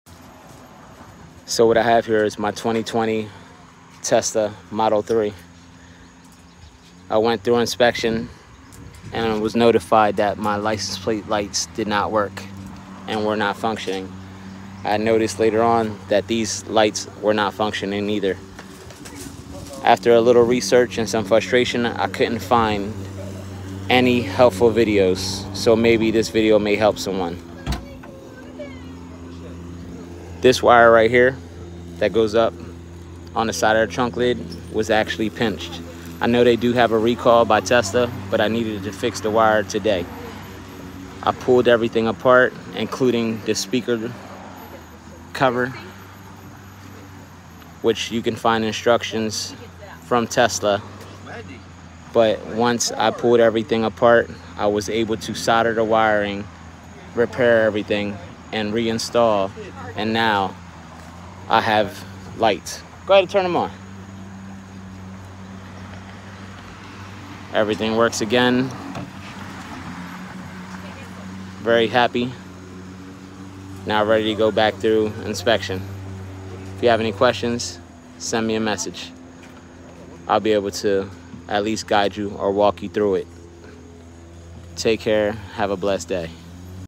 2020 Tesla model 3 trunk and license plate lights not coming on. Here’s a quick tip.
Hello this is my 2020 Tesla model  when I purchased this vehicle I did not noticed that my trunk tail lights and license plate lights were not coming on before having the car inspected now I have to fix it. I did some research and seeing that Tesla did have a recall, but after putting in my VIN number, the recall did not come up from my vehicle so I dove right in I try to find other videos or some helpful information that I could use, but I couldn’t find anything useful. So I started to trace the wires and found that they were under the rear deck lid / panel where the speaker are installed. Once I removed all the necessary parts. I pulled out the wiring harness from the side of the trunk and start to tug on each wire one at a time and found that the tan wire inside. The send it to me wiring harness was broken. That was also two other wires and orange and a purple that we’re starting to break. After adding some wire and soldering everything back together, Walla, everything works like new for free just my time it took me around 35-40 mins to do the complete job.